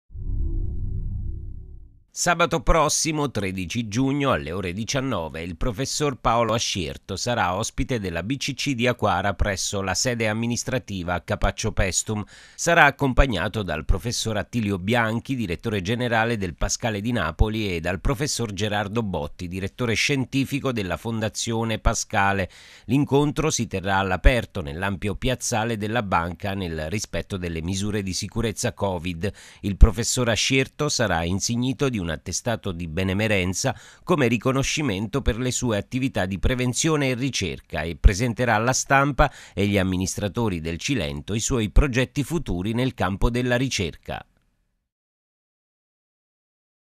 Capaccio Paestum: il 13 giugno Paolo Ascierto ospite della BCC di Aquara
Sabato 13 giugno p.v., alle ore 19,00, il Prof. PAOLO ASCIERTO sarà ospite della BCC di Aquara presso la nostra Sede Amministrativa a Capaccio. Sarà accompagnato dal Prof. Attilio Bianchi , Direttore Generale del Pascale di Napoli, e dal Prof. Gerardo Botti, Direttore Scientifico della Fondazione Pascale.